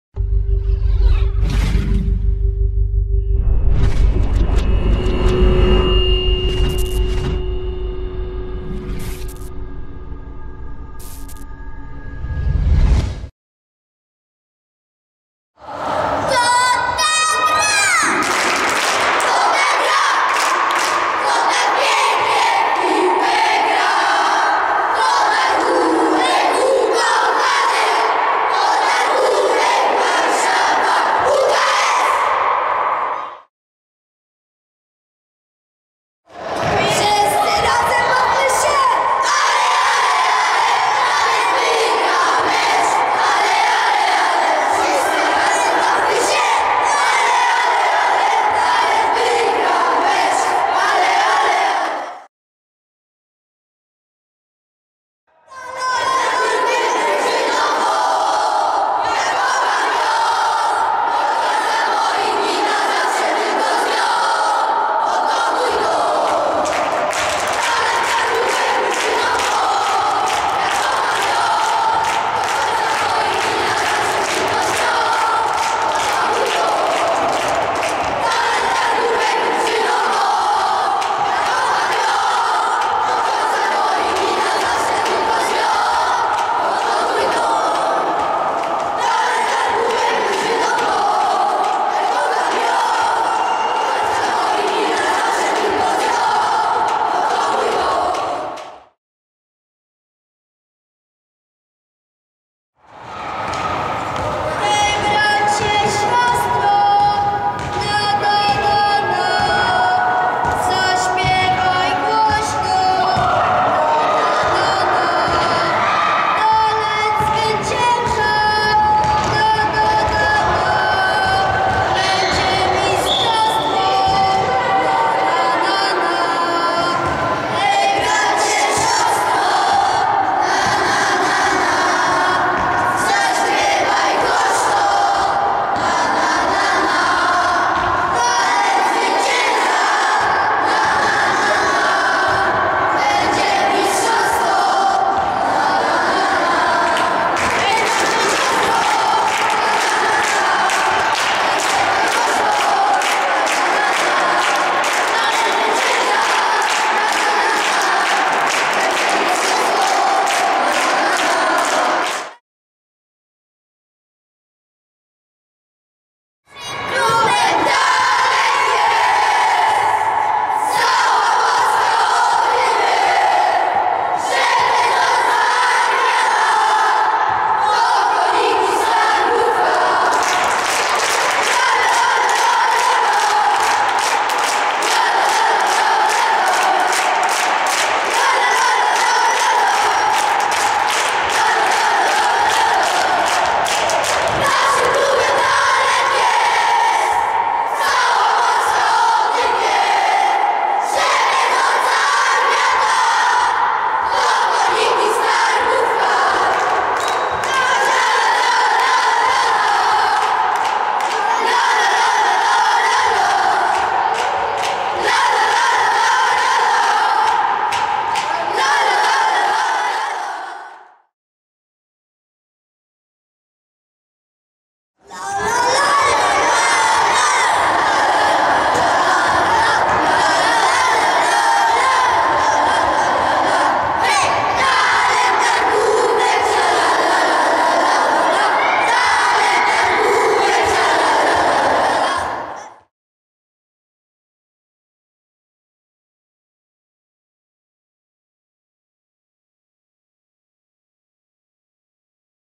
Talent Targówek TV: Śpiewnik
-Kto tak gra?
-Kto tak gra?
-Kto tak pięknie w piłkę gra?
-To nasz Talent ukochany!
-To nasz Talent Warszawa! UKS!

-Wszyscy razem bawmy się!
-Allez, allez, allez
-Talent wygra mecz!

-Talent Warszawa drużyną mą,
-Ja kocham ją!
-Do końca moich dnia na zawsze tylko z nią!
-Do to mój dom!

-Hej bracie, siostro!
-Na na na na
-Zaśpiewaj głośno
-Talent zwycięża
-Będzie mistrzostwo!

-Naszym klubem Talent jest!
-Cała Polska o tym wie!
-Że pędząca armia ta.
-To koniki z Targówka!